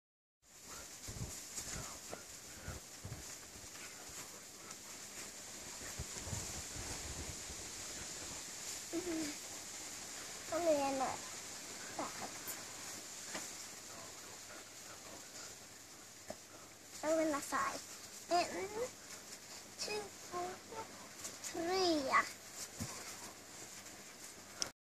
Lovis tränar
Lovis kör barn/lekträning i trädgården